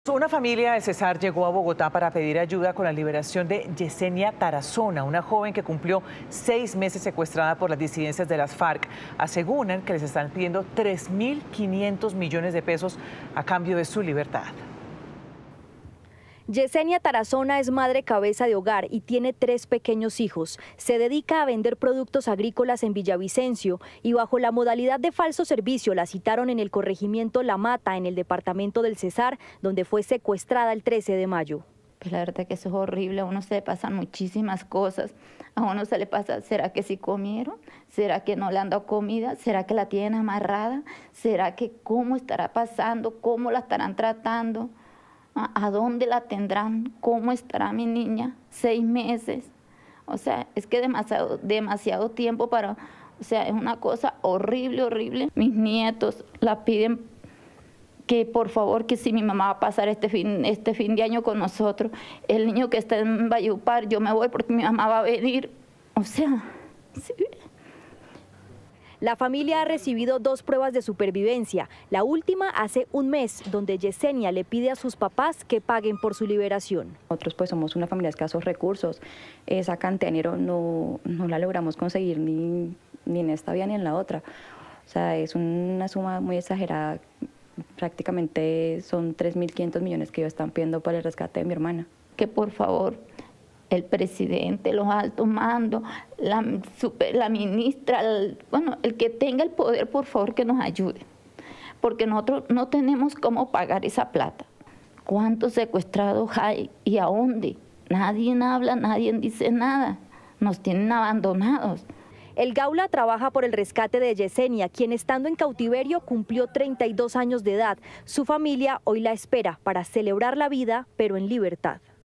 Mujer lleva 6 meses secuestrada por las disidencias de las FARC, que piden $3.500 millones
Yesenia Tarazona es una madre cabeza de hogar que tiene tres pequeños hijos. Se dedica a vender productos agrícolas en Villavicencio, pero bajo la modalidad de falso servicio la citaron en el corregimiento La Mata, en el departamento de Cesar, donde fue secuestrada el 13 de mayo de 2023.

Ángela Andrade, madre de esta joven, afirma que no ha tenido tranquilidad desde ese momento: “¿Será que sí comió? ¿Será que la tienen amarrada? ¿Cómo estará pasando? ¿Cómo la estarán tratando? ¿Cómo estará mi niña? Seis meses es demasiado tiempo, es una cosa horrible”.

La mamá de la mujer secuestrada en Cesar le pidió “al presidente, a los altos mandos que tengan el poder que por favor nos ayuden, no tenemos cómo pagar esa plata”.